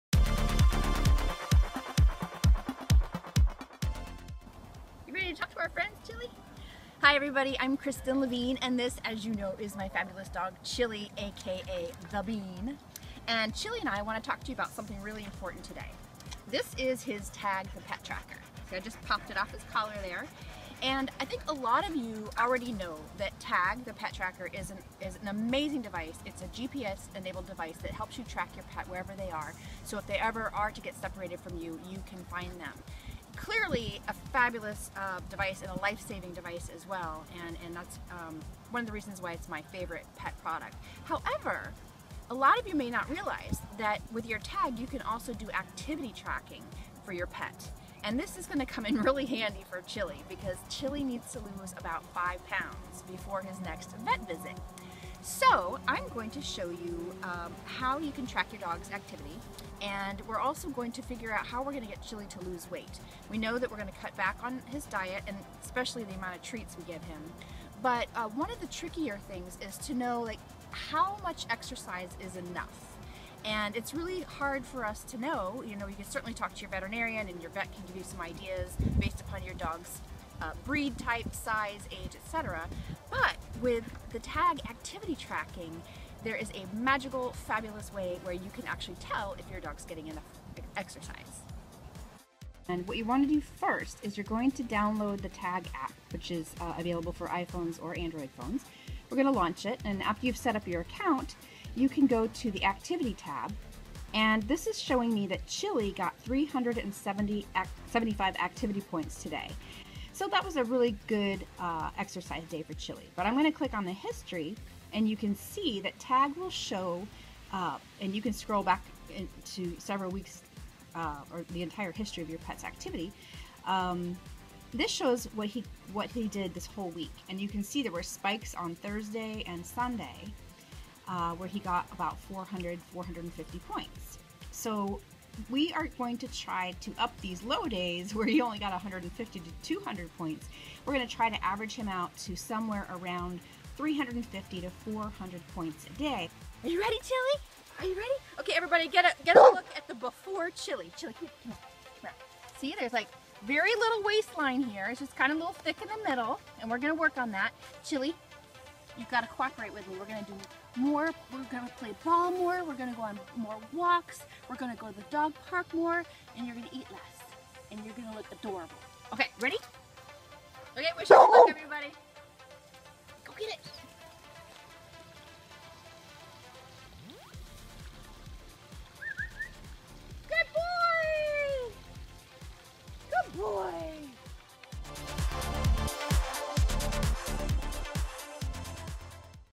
Chilly Fights His 'Battle Of The Bulge' With Tagg's Activity Tracking Feature (HD)
Kristen Levine, Pet Lifestyle Expert and her dog Chilly shot this video to show you how they will put Tagg to work measuring Chilly's daily exercise as part of their recipe for weight loss success.